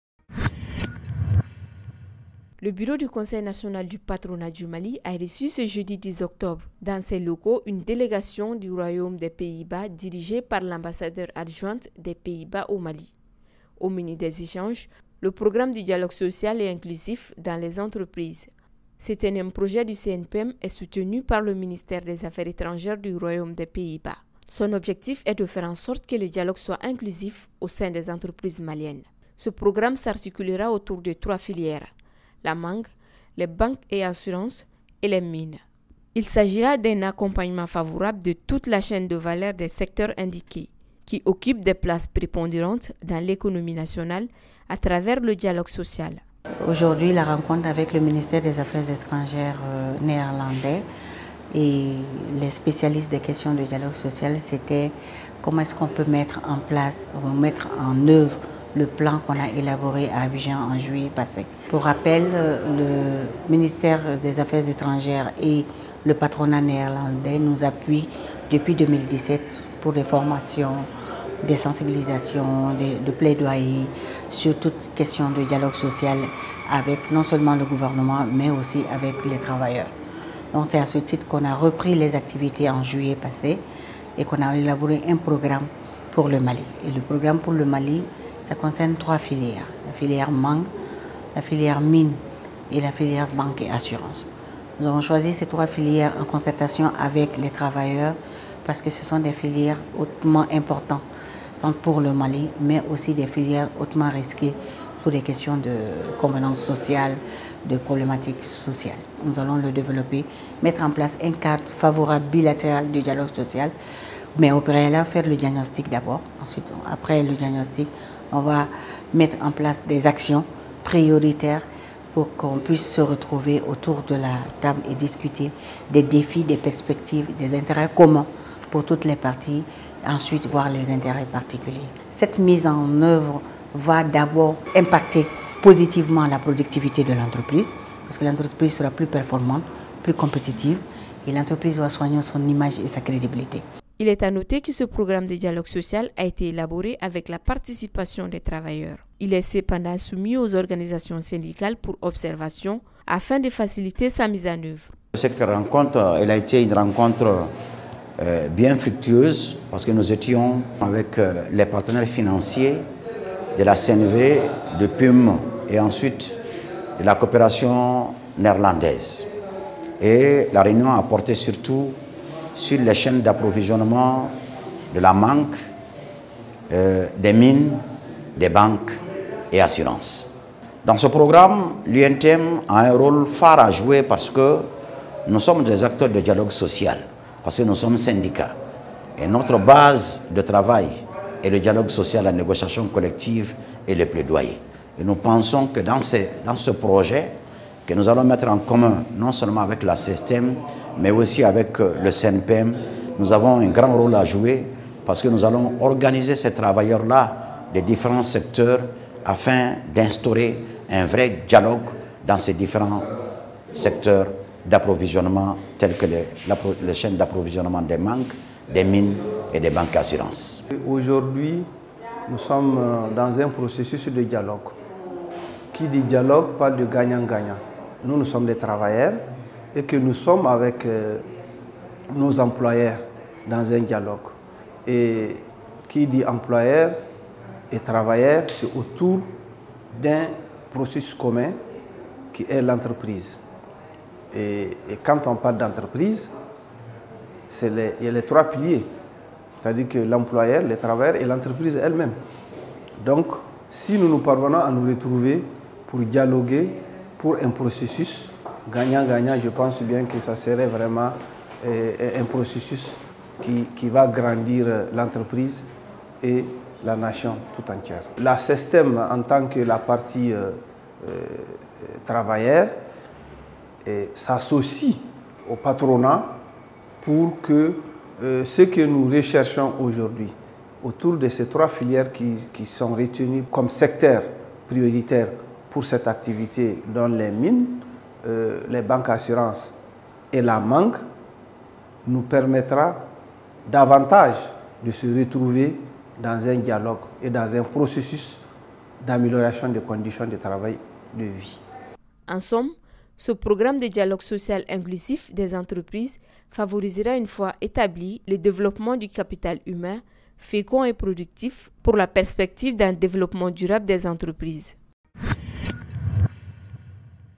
UNE DÉLÉGATION DU ROYAUME DES PAYS-BAS REÇUE PAR LE BUREAU DU CNPM
🔴 Le bureau du Conseil national du Patronat du Mali (CNPM) a reçu dans ses locaux une délégation du Royaume des Pays-Bas dirigée par l’ambassadeur adjointe des Pays-Bas au Mali. Au menu des échanges : le programme du dialogue social et inclusif dans les entreprises.
©mali-online.net